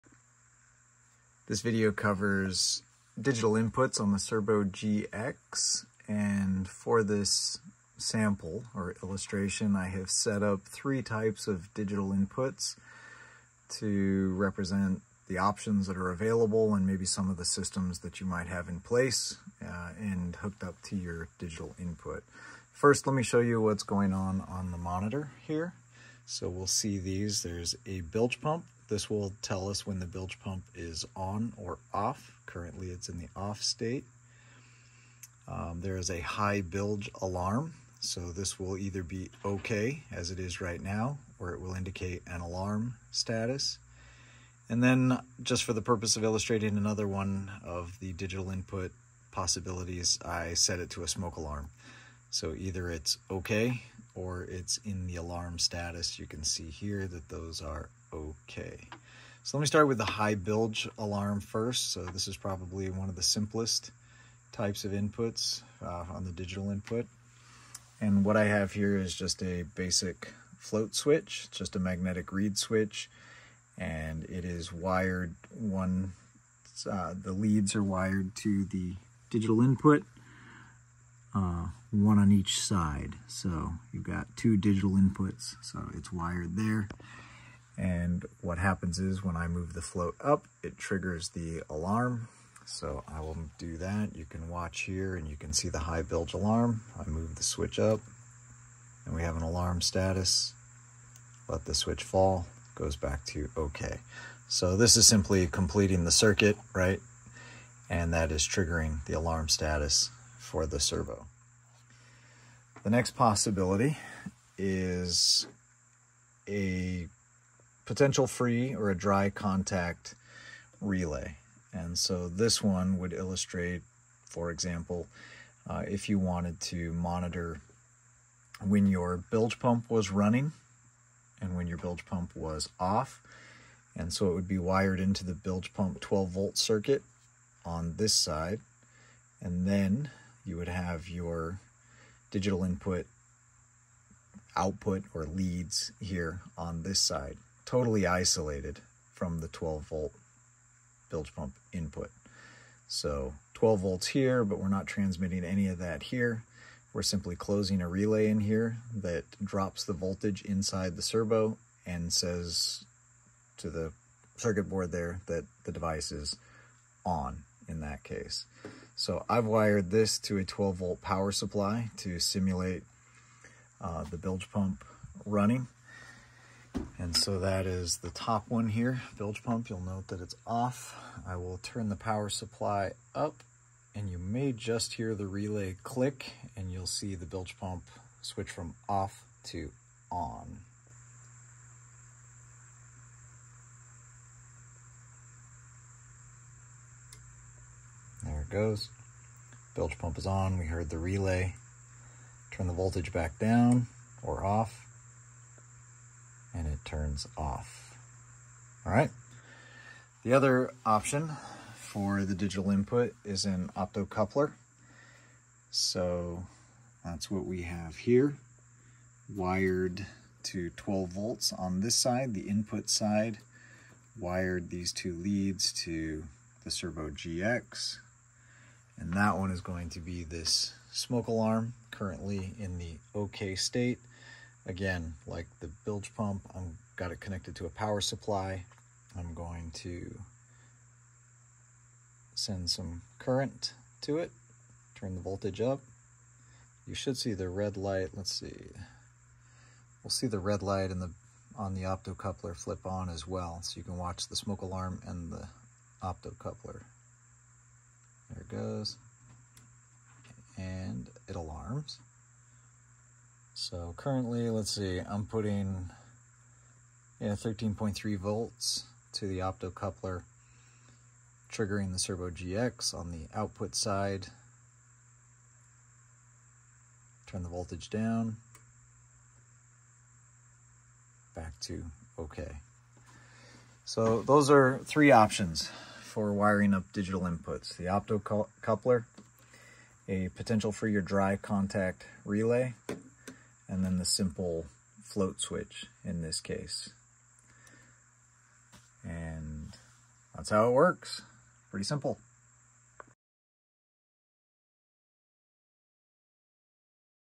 Victron Cerbo GX Digital Input Options
A brief explanation of wiring three different types of digital inputs on the Victron Cerbo GX: magnetic reed switch, potential-free or dry contact relay, and an optocoupler.